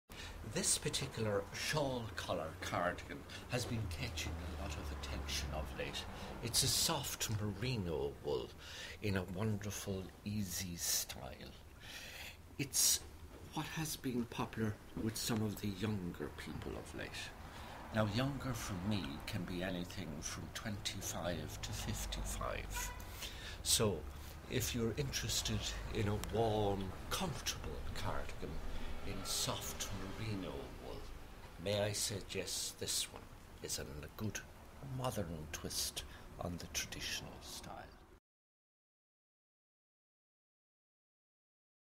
Aran Sweater Pattern
- we've got every aran sweater pattern you could imagine in our selection of jumpers, sweaters and cardigans at Irish Handcrafts. Call in and meet Michael who will happily explain the intricacies and history of the famous Aran sweater. And he'll happily sell you one too :). There's a pattern to his madness, but - no sweat! - because when you try on one of these magnificient garments you'll be sold anyway.